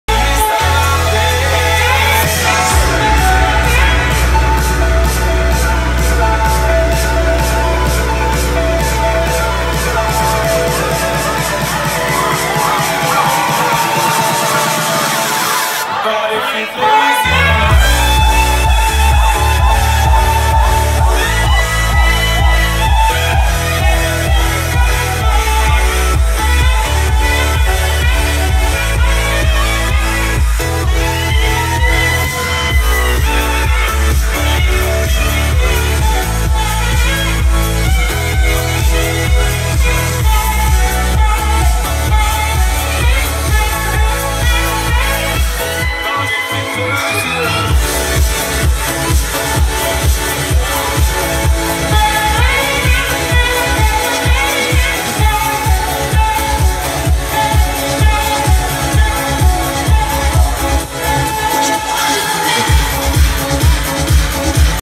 Saxo & DJ
Saxo en vivo en fiesta de matrimonio acompañado por el DJ en la pista de baile, Chile